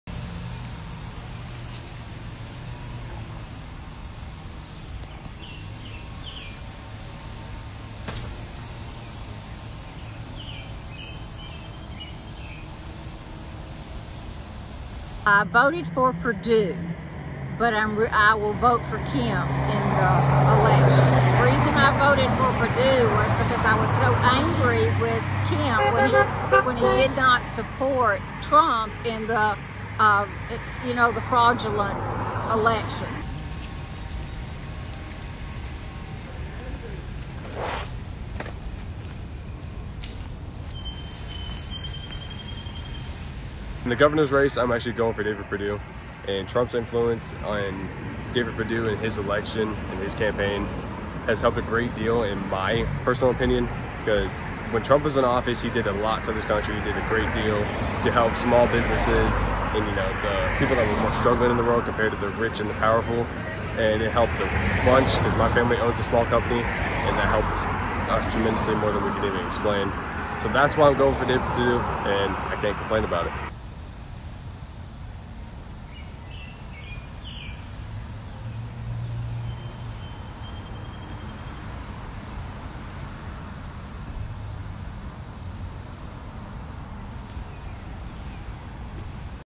Voting underway in Georgia primary election
RESTRICTION SUMMARY:
ASSOCIATED PRESS
Decatur, Georgia - 24 May 2022
1. Exterior of voting facility
ASSOCIATED PRESS
Woodstock, Georgia - 24 May 2022
2. SOUNDBITE (English) Pam Leonard, Woodstock, Georgia resident:
"I voted for Perdue, but I will vote for Kemp in the election. Reason I voted for Perdue was because I was so angry with Kemp when he did not support Trump in the, you know, the fraudulent election."
ASSOCIATED PRESS
3. Exterior of voting facility
ASSOCIATED PRESS
4. SOUNDBITE (English) Brody Nelson, Dallas, Georgia resident:
"In  the governor's race. I'm actually going for David Perdue and Trump's influence on David Perdue in his election, in his campaign, has helped a great deal, in my personal opinion, because when Trump was in office, he did a lot for this country. He did a great deal to help small businesses. And, you know, the people that are struggling in the world compared to the rich and the powerful. And it helped a bunch because my family owns a small company and that helped us tremendously more than we can even explain. So that's why we go for David Perdue and I can't complain about it."
ASSOCIATED PRESS
5. Pan across voting facility
STORYLINE:
Georgia takes center stage in Tuesday's primary elections as Gov. Brian Kemp and Secretary of State Brad Raffensperger try to fight back challengers endorsed by Donald Trump.
Congresswoman Marjorie Taylor Greene of Georgia is testing Republican voters' tolerance for controversy in her primary.
In Arkansas, former White House press secretary Sarah Sanders is a front-runner for the Republican nomination for governor.
In Alabama, three Republicans are in a tight race for the nomination to replace retiring U.S. Sen. Richard Shelby.
In two Texas runoffs, Attorney General Ken Paxton is trying to hold off Land Commissioner George P. Bush, while congressman Henry Cuellar is facing a progressive challenger.